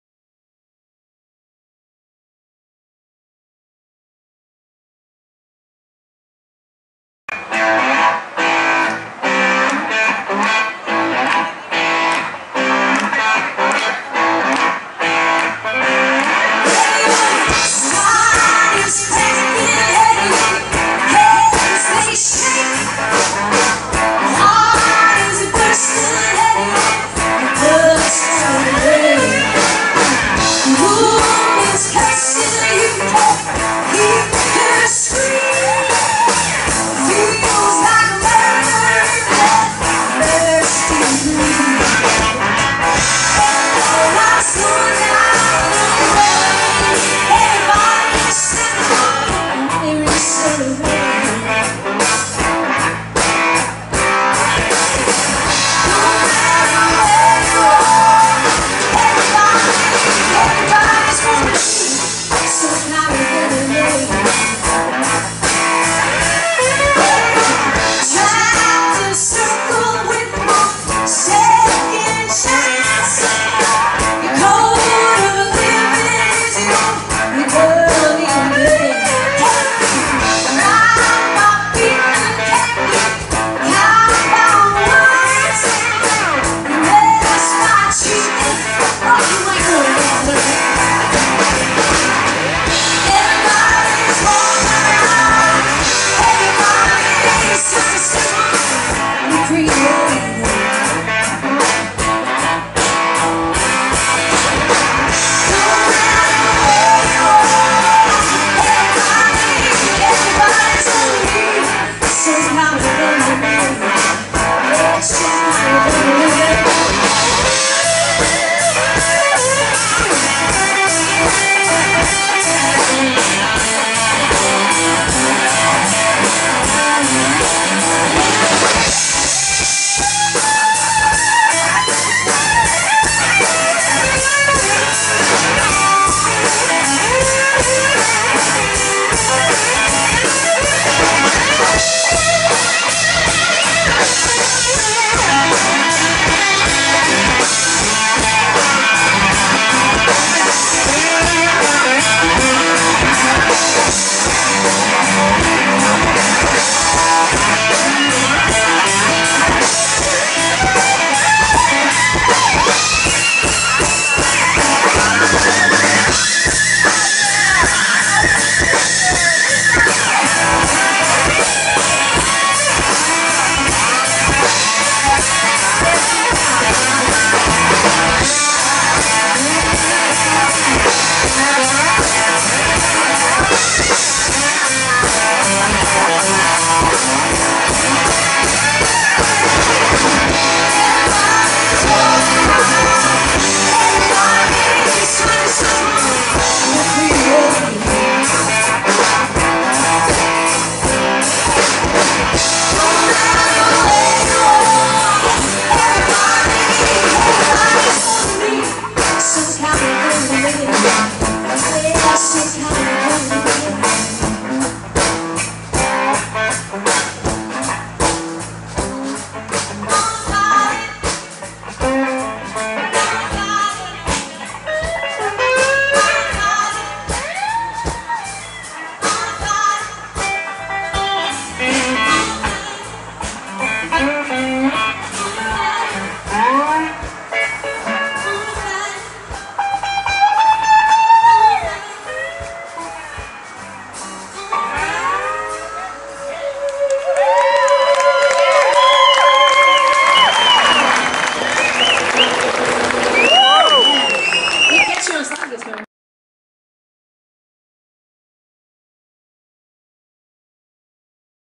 NICK BOETTCHER & PAIGE DeCHAUSSE ILLINOIS BUDWEISER BLUES FEST 2015 LIVE
Peoria Blues Fest !